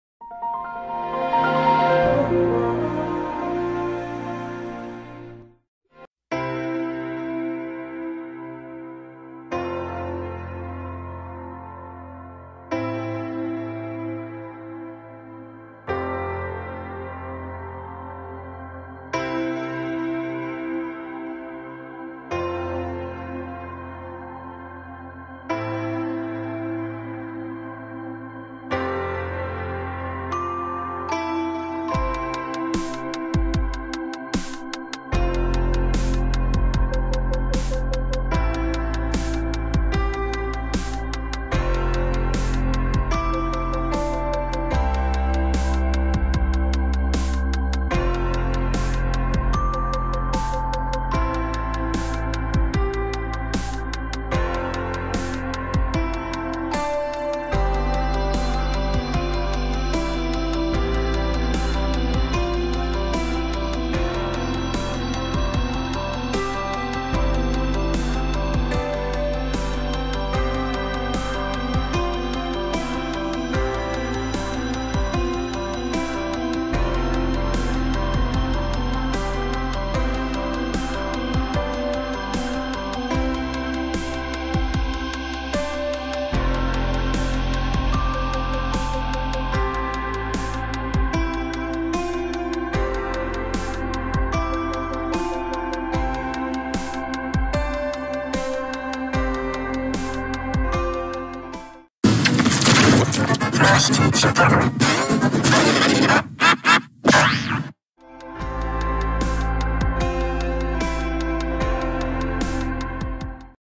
How To Make G-Major -1 On Sony Vegas Pro
Made on Sunday, April 21, 2019, 11:05:20 pm to 11:06:44 pm

Let's make G-Major -1 effect on Sony Vegas Pro.

Upload Time: May 12, 2019, 3:13 pm